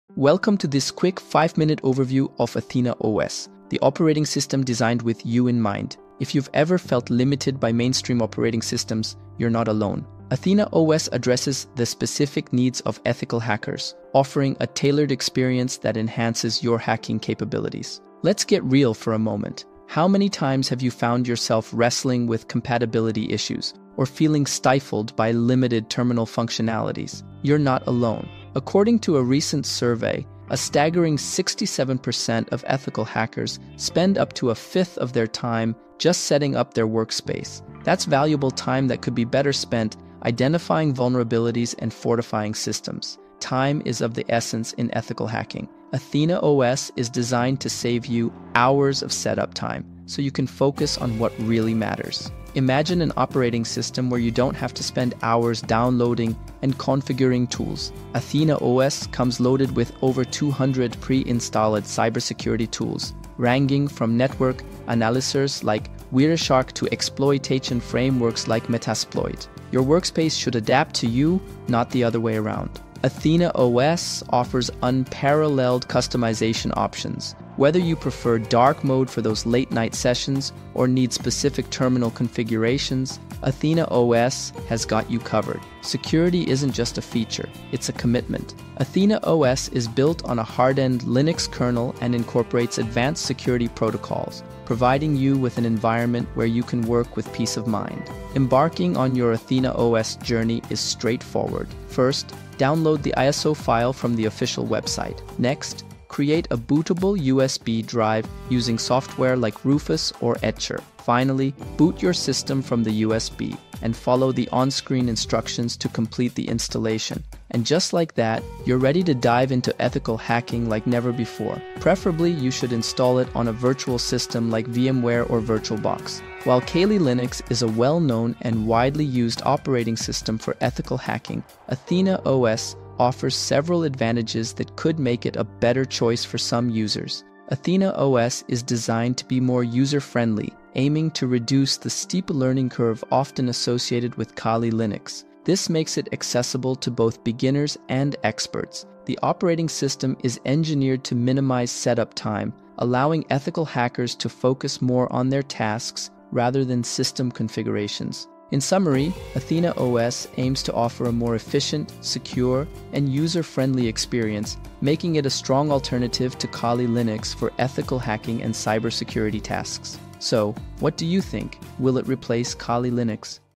Athena OS: The Ultimate OS for Ethical Hackers?
Discover Athena OS, the operating system designed for ethical hackers. Learn about its key features, benefits, and how to get started.

Unlock the full potential of ethical hacking with Athena OS, the ultimate operating system designed for cybersecurity enthusiasts. Say goodbye to compatibility issues and time-consuming setups. Athena OS comes pre-loaded with essential hacking tools and offers a customizable, secure environment that sets it apart from Kali Linux.

Join our growing community to elevate your ethical hacking skills, streamline your workflow, and enhance your system's security.